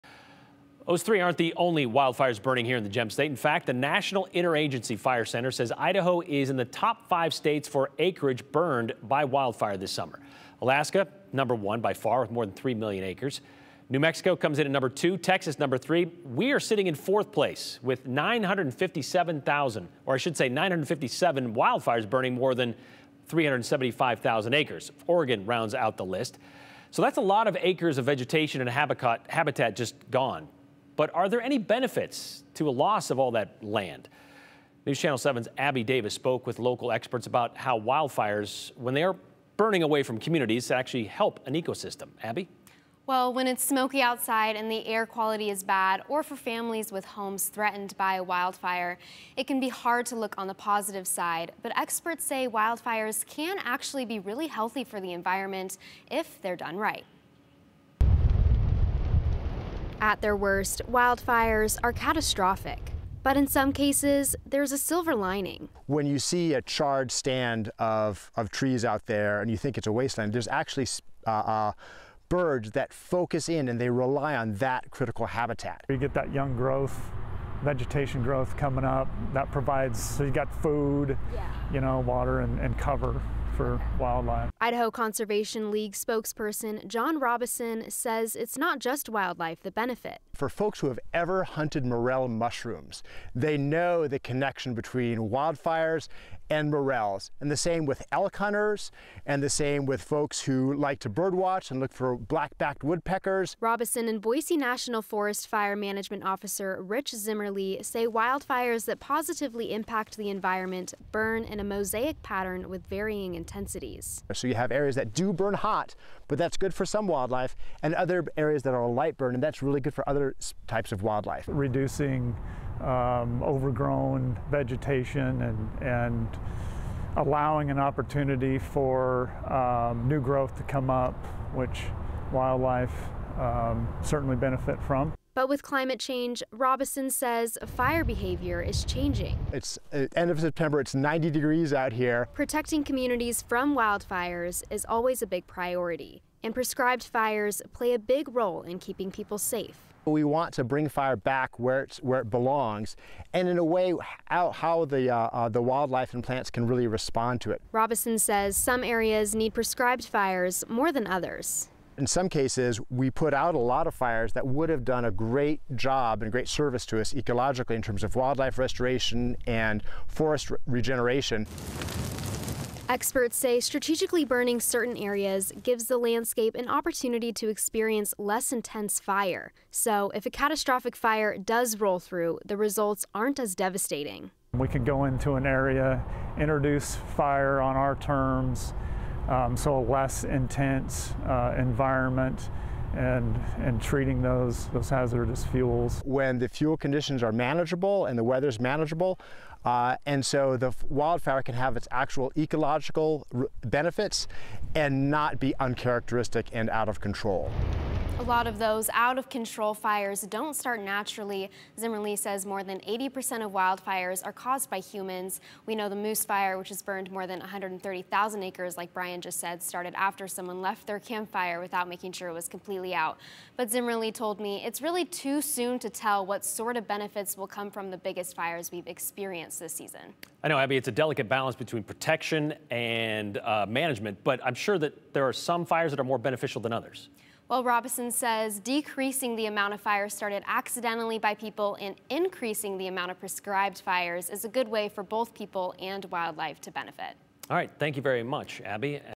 Wildfires can benefit various species, overall ecosystem
When wildfires are not catastrophic, there are several benefits – some species even thrive.

“When you see a charred stand of trees out there and you think it's a wasteland, there's actually birds that focus in and they rely on that critical habitat,” Idaho Conservation League Public Lands Director, John Robison said.